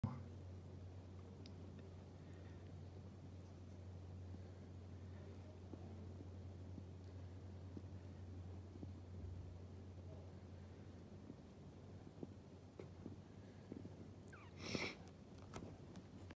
Biggest tick ever
I found this bastard om me...